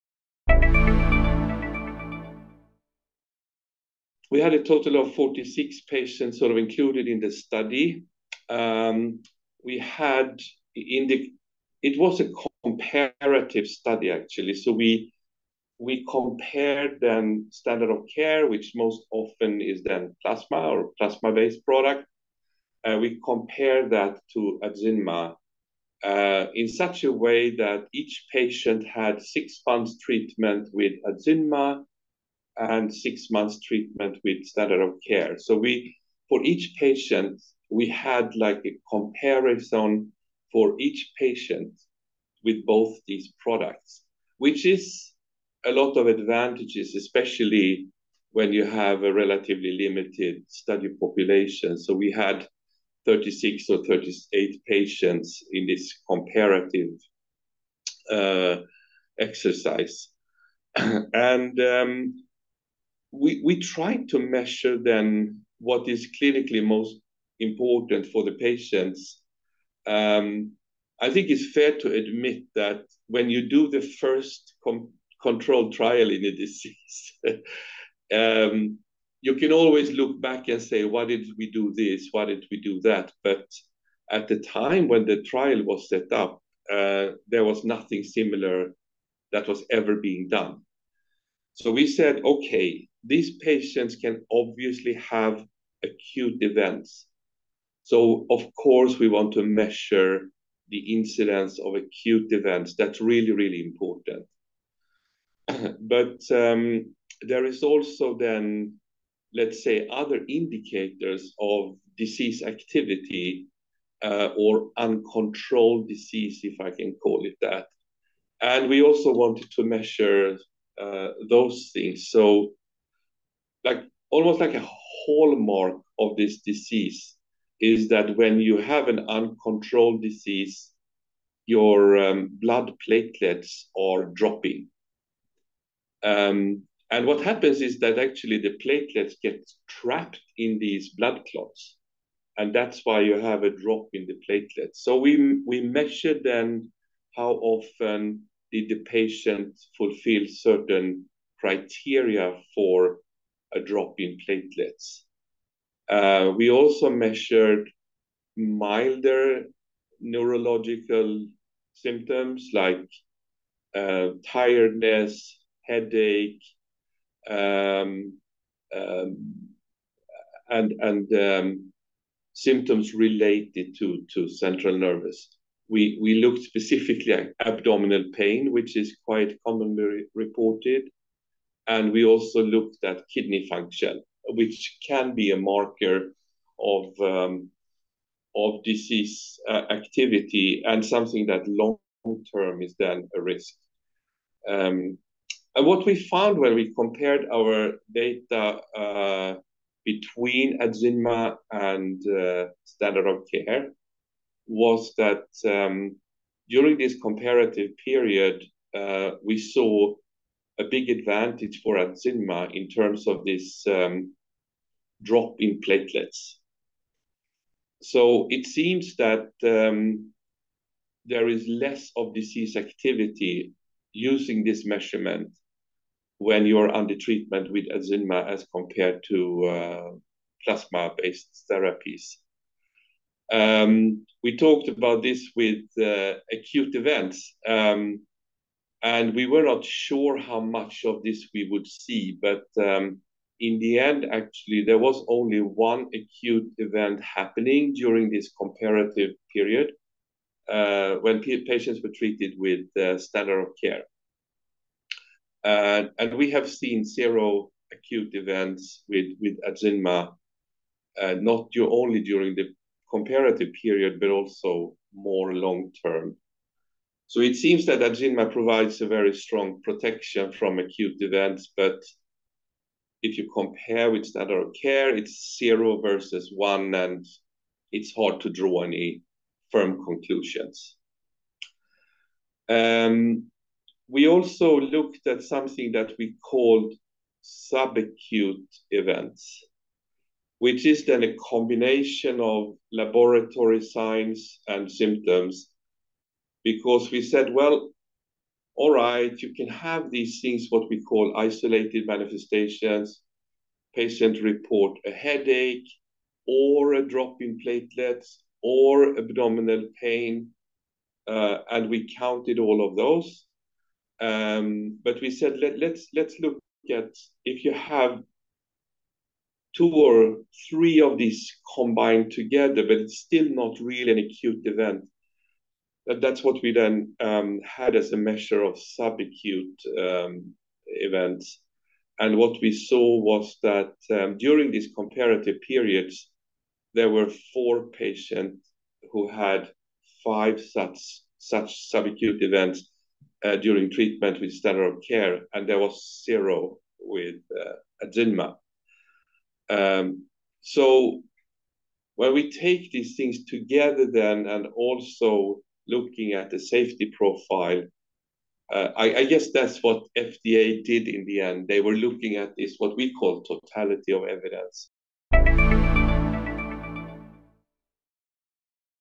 Adzynma's Phase 3 Clinical Trial and Approval
Björn Mellgård, M.D., Ph.D., Vice President, Global Program Lead, Rare Genetics and Hematology, Takeda, discusses the steps taken for ADZYNMA to get FDA approved.

Transcription:
We had a total of 46 patients included in the study. It was a comparative study. We compared then the standard of care, which most often is plasma or plasma-based product. We compared that to ADZYNMA, in such a way that each patient had six months treatment with ADZYNMA and six months treatment with standard of care.

For each patient, we had a comparison for each patient with both these products, which has a lot of advantages, especially when you have a relatively limited study population. We had 36 or 38 patients in this comparative exercise, and we tried to measure then what is clinically most important for the patients. I think it's fair to admit that when you do the first controlled trial in a disease, you can always look back and say, "Why did we do this? Why did we do that?" But at the time when the trial was set up, there was nothing similar to that was ever being done.

We said, "Okay, these patients can obviously have acute events." So of course, we want to measure the incidence of acute events. That's really important. But there are also, let's say, other indicators of disease activity, or uncontrolled disease, if I can call it that. We also wanted to measure those things.

Almost like a hallmark of this disease, when you have an uncontrolled disease, your blood platelets are dropping. What happens is that the platelets get trapped in these blood clots, and that's why you have a drop in the platelets. We measured how often the patient fulfills certain criteria for a drop in platelets. We also measured milder neurological symptoms like tiredness, headache, and symptoms related to central nervousness. We looked specifically at abdominal pain, which is quite commonly reported. We also looked at kidney function, which can be a marker of disease activity, and something that long term is a risk.

What we found when we compared our data, between ADZYNMA and standard of care, was that during this comparative period, we saw a big advantage for ADZYNMA in terms of drop in platelets. It seems that there is less disease activity using this measurement when you are under treatment with ADZYNMA as compared to plasma-based therapies. We talked about this with acute events, and we were not sure how much of this we would see, but in the end, there was only one acute event happening during this comparative period when patients were treated with standard of care. We have seen zero acute events with ADZYNMA, not only during the comparative period, but also in the long term.

So, it seems that ADZYNMA provides a very strong protection from acute events. But if you compare it with standard care, it's zero versus one, and it's hard to draw any firm conclusions.

We also looked at something we called subacute events, which is then a combination of laboratory signs and symptoms. Because we said, you can have these things, what we call isolated manifestations, patient report a headache, a drop in platelets, or abdominal pain, and we counted all of those. But we said, "If you have two or three of these combined together, it's still not really an acute event." That's what we then had as a measure of subacute events. What we saw during this comparative period, there were four patients who had five such subacute events during treatment with standard of care, and there was zero with ADZYNMA.

When we take these things together, and look at the safety profile, I guess that's what FDA did in the end. They were looking at this, what we call totality of evidence.